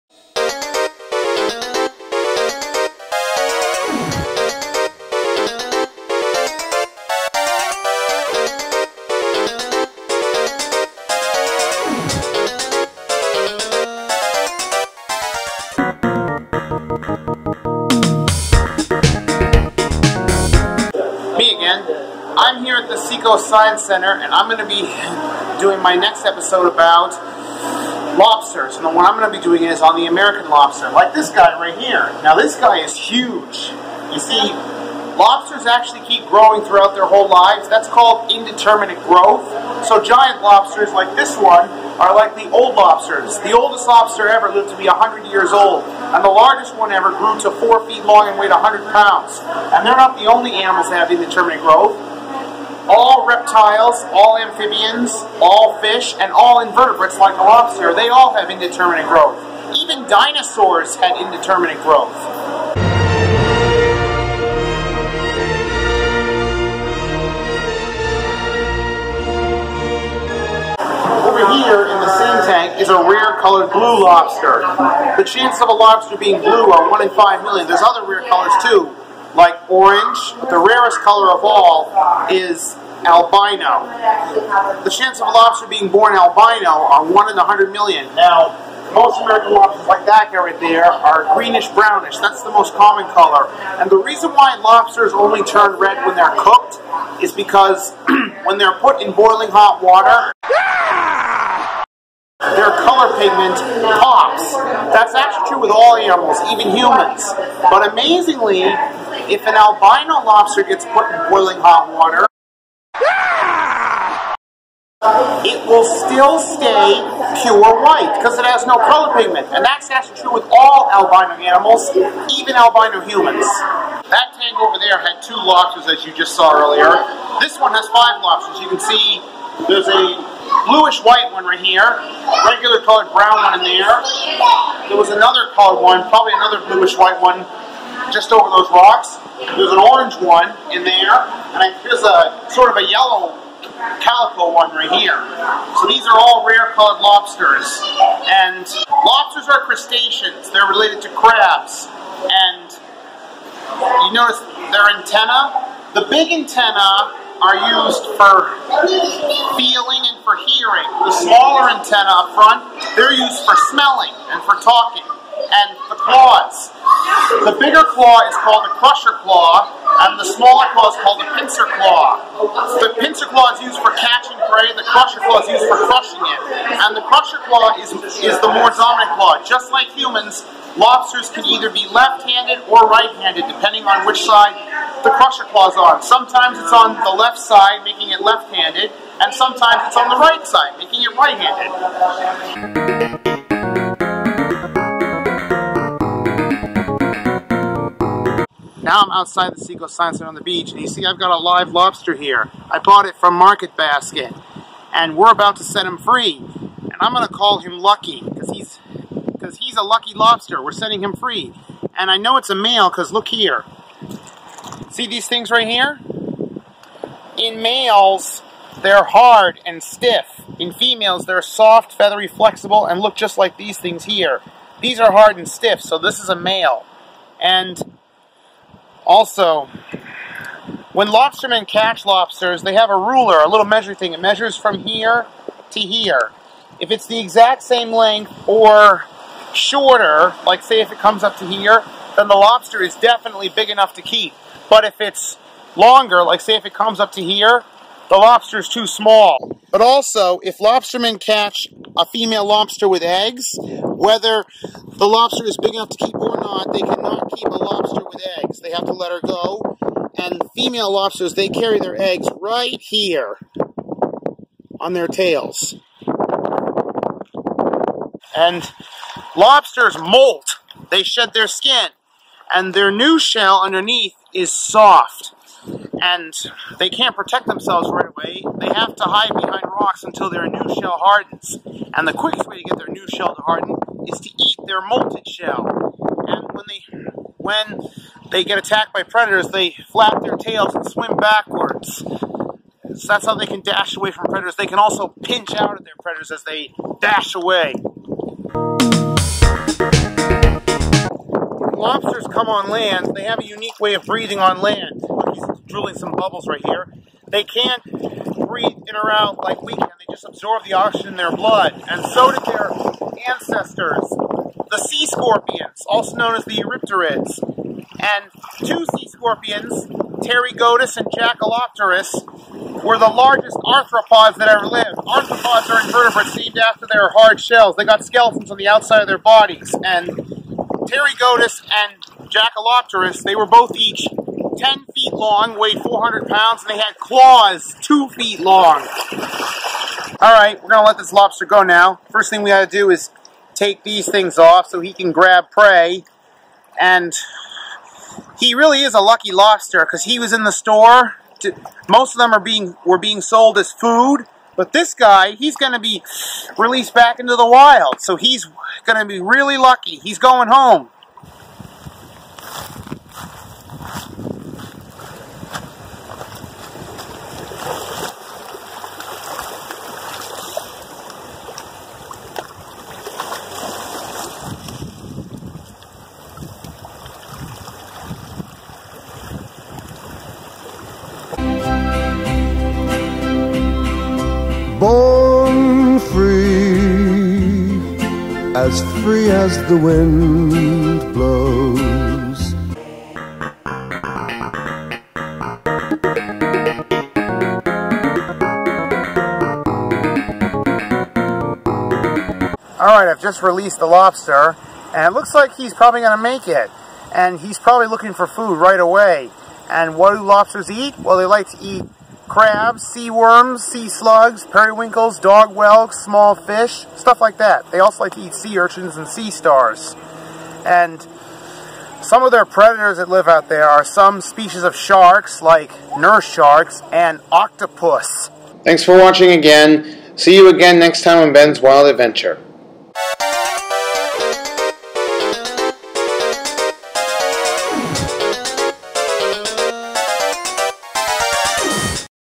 Ben's Wild Adventure: Episode 15 - The Lobster
I hope you enjoy this fifteenth episode of Ben's Wild Adventure-- a show where I talk about my number one topic, animals!  I love animals, and I would like to share the fun facts I know about them with all of you.